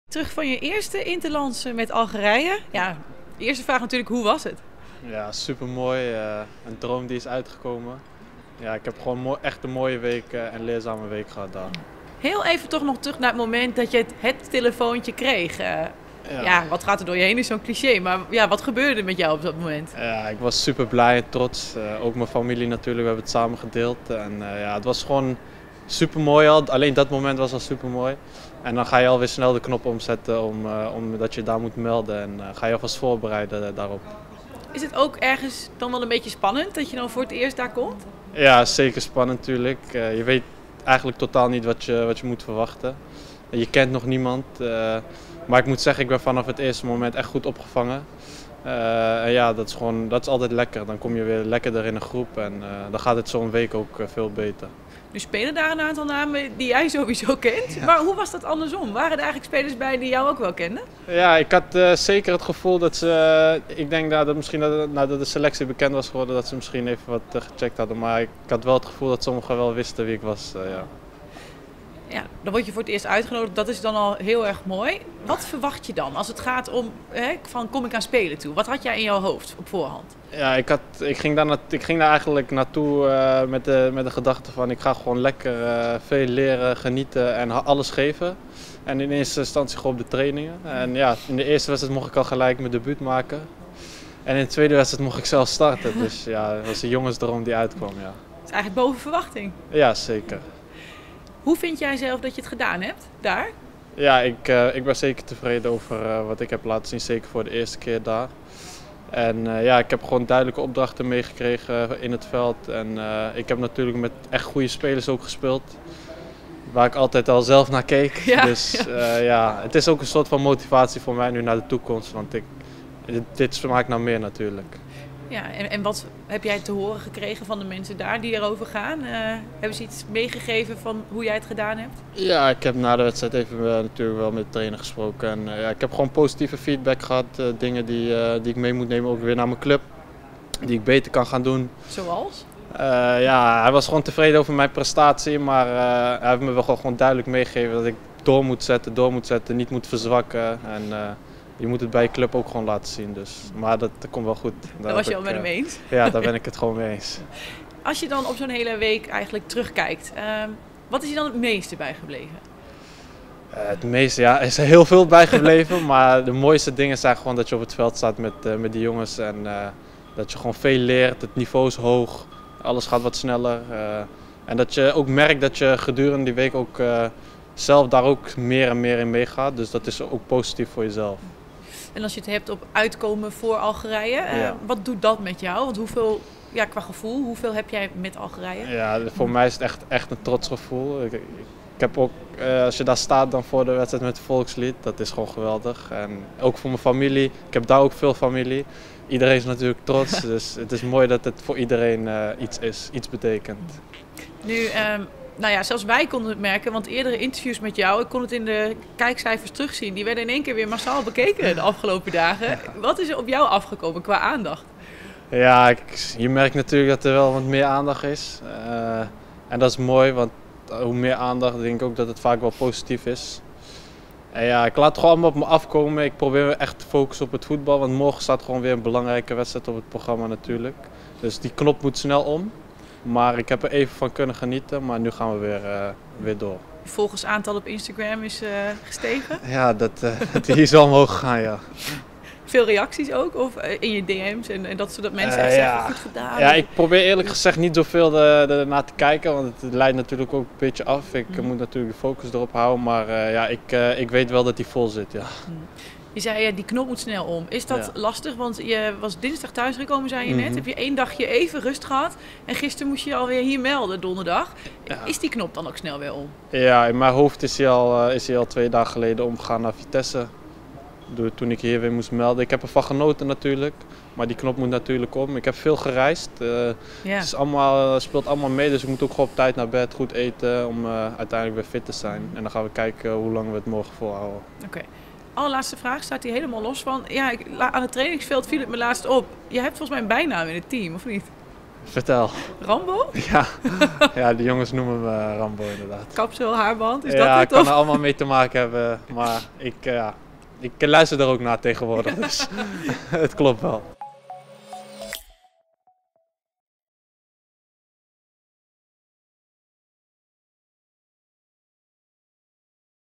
Zerrouki trots na debuut bij Algerije, maar de knop is alweer om naar Vitesse
De goede prestaties van Ramiz Zerrouki bij FC Twente zijn niet onopgemerkt gebleven. Onlangs ontving hij zijn eerste uitnodiging voor het nationale elftal van Algerije dat de afgelopen week kwalificatiewedstrijden voor de Afrika Cup speelde tegen Zambia en Botswana. Tegen Zambia kon Zerrouki al rekenen op een invalbeurt, tegen Botswana wachtte meteen een basisplaats.

Dinsdag keerde hij terug in Nederland en donderdag meldde hij zich weer bij FC Twente, waarmee zaterdag alweer de thuiswedstrijd tegen Vitesse wacht. En dus moet de knop weer om.